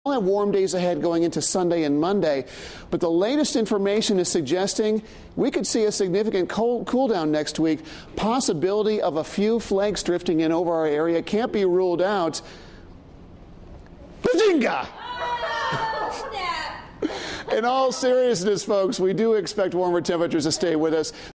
Is Snow Coming Back Into The Forecast?
Brian Slocum gives his April Fool's forecast and almost got the morning crew this morning...